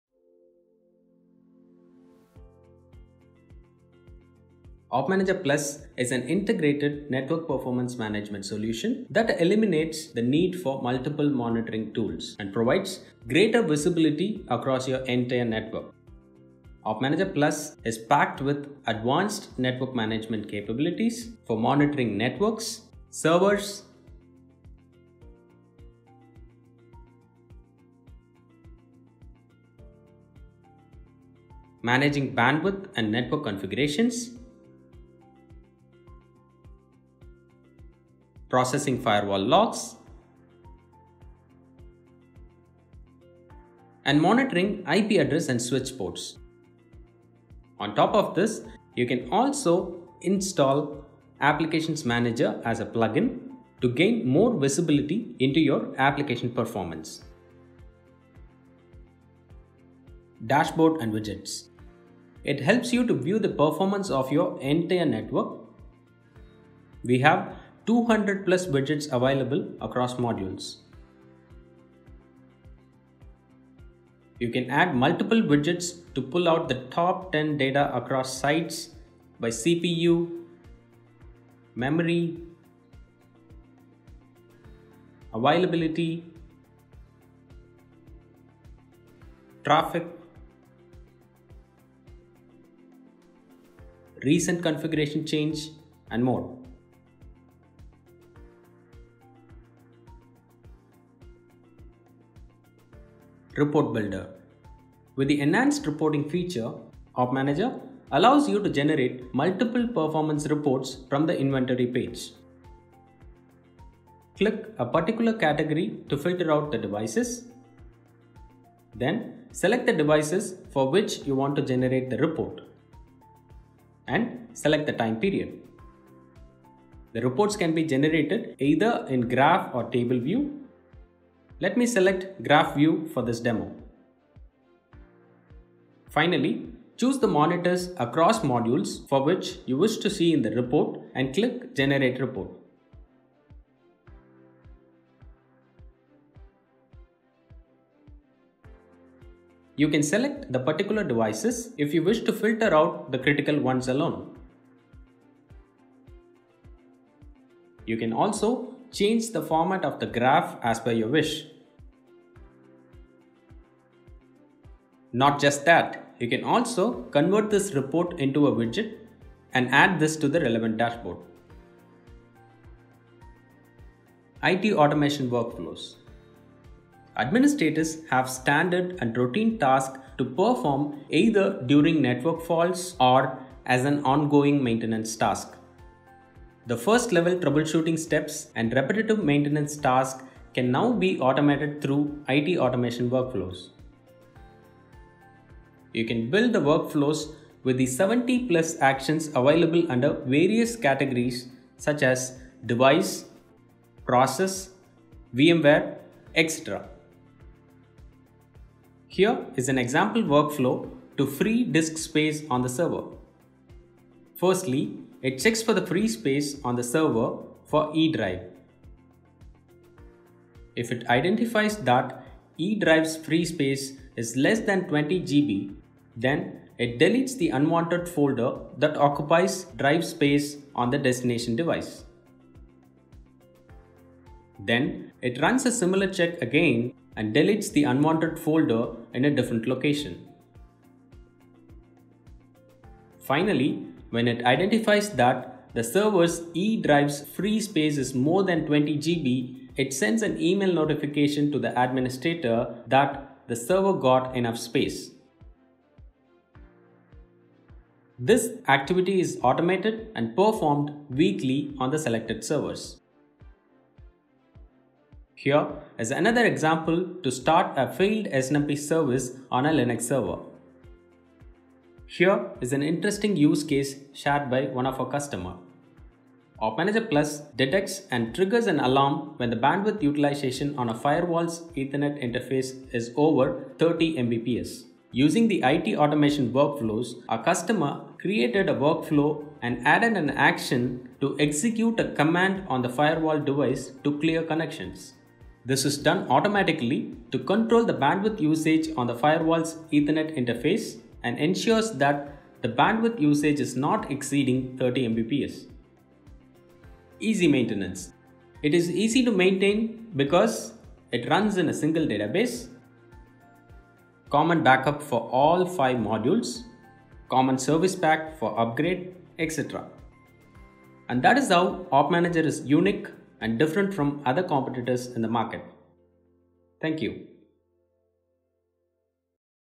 Top Features of OpManager Plus | Add-ons & WorkFlows
With the launch of OpManager Plus, we have upped the level of network monitoring by integrating many useful tools.

This video discusses the 'Top features of OpManager Plus' which can play a vital role in monitoring your network.

# It removes the need for external tools and provides greater visibility over your network.
# Applications Manager addon is available.
# New Dashboard and Widget which allows you to pull top data based on CPU, Memory, Availability, etc.

Apart from these, efficient report builder, IT automation workflows and easy maintenance are other important attributes of the product.